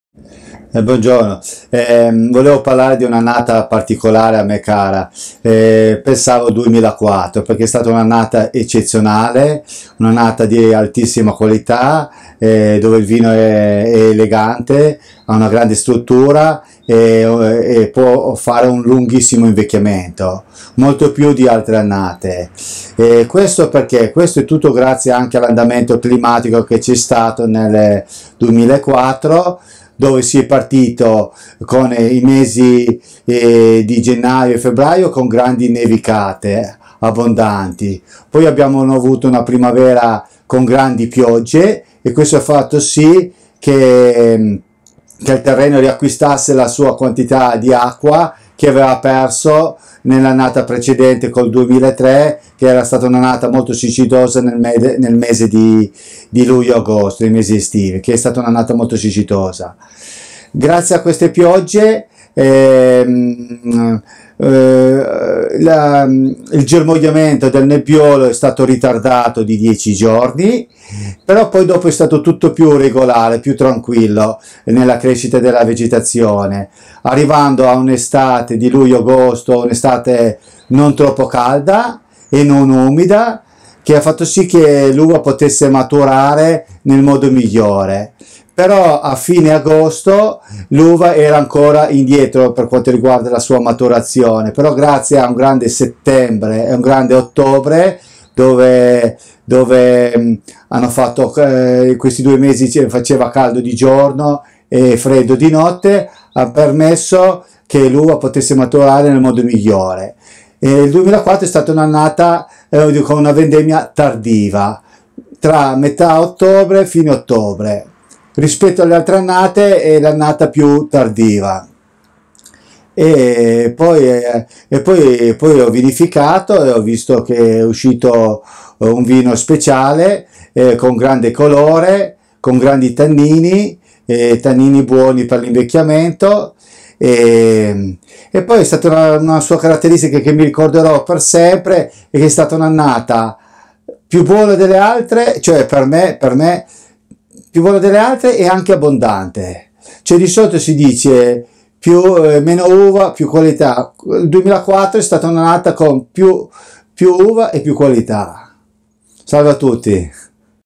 "A Vintage in Barbaresco" ... by Giuseppe Marengo
"I want to tell you about the 2004 vintage, an exceptional year, which gave us wines which have a very long aging ... ", tells Giuseppe Marengo, who manage with his sisters Paola and Maria the Ca' Rome' wine estate, founded by the father Romano Marengo.